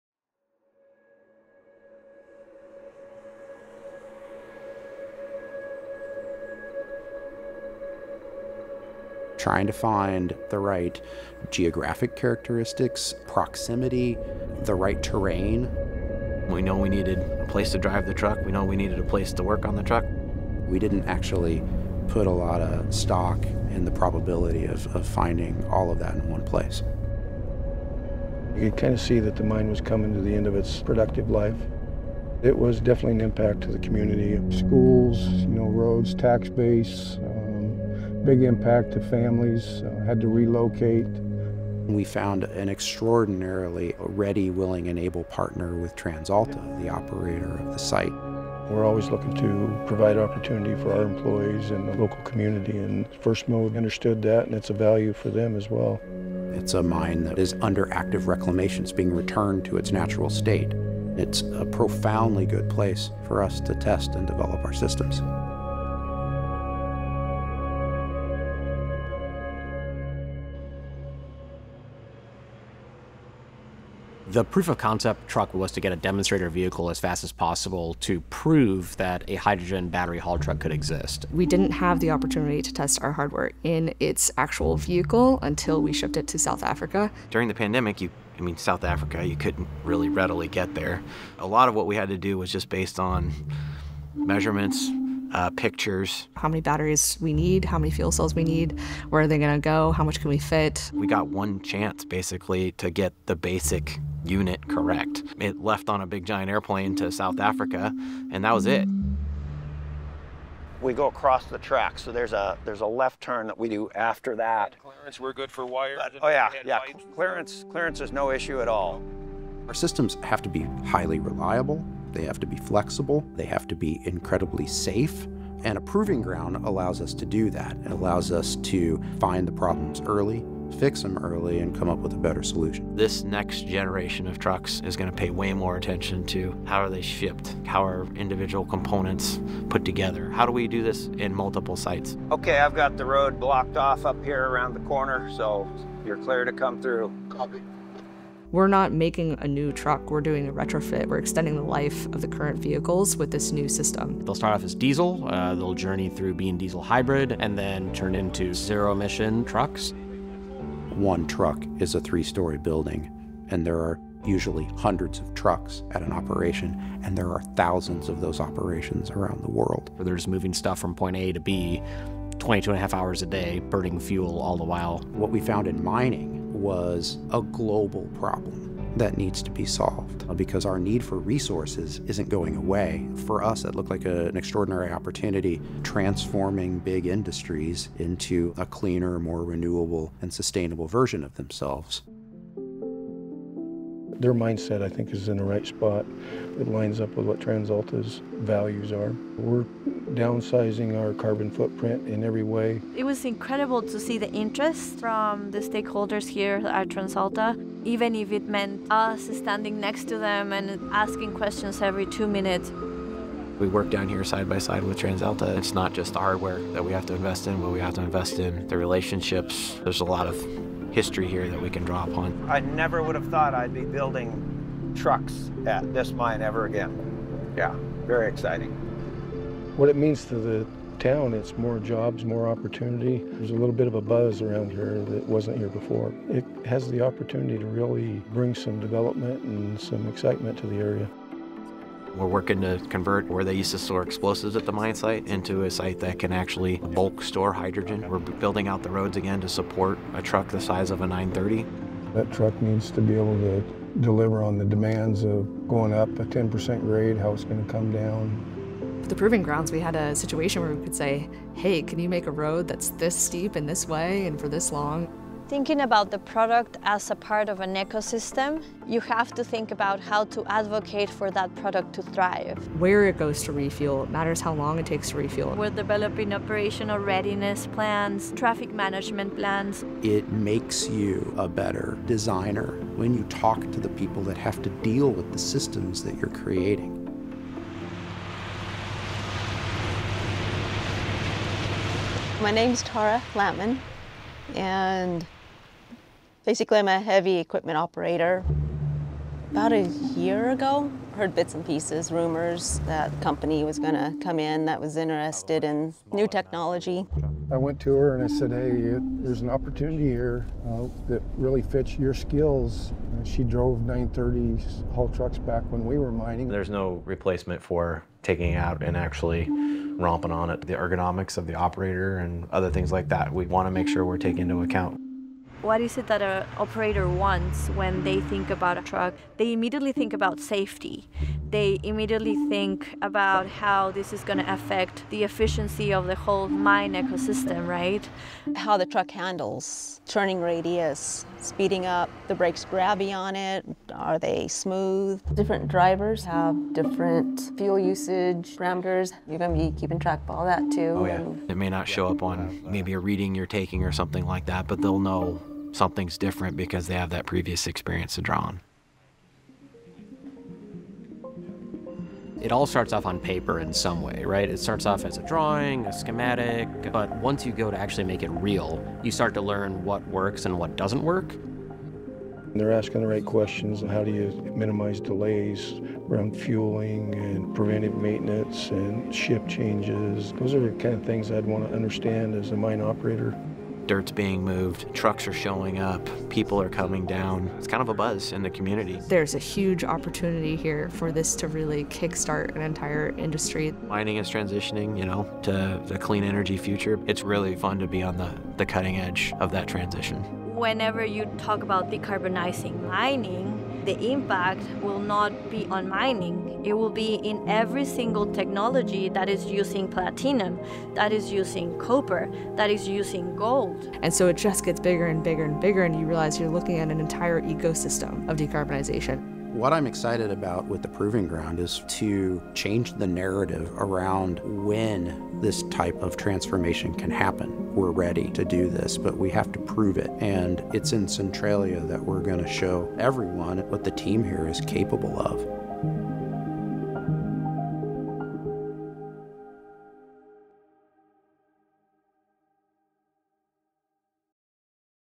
Revolutionizing Heavy Industry: Inside Look at the First Mode Proving Grounds
In Autumn 2022 we announced the First Mode Proving Grounds, a first-of-its-kind facility for testing, demonstrating, and commercializing zero-emission solutions for ultra-class haulage. Since then, we’ve been hard at work building out the facility to provide a real-world testing environment for our nuGen™ Haulage Solutions.

For more on how First Mode is using innovative clean energy technology solutions to decarbonize heavy industry, visit firstmode.com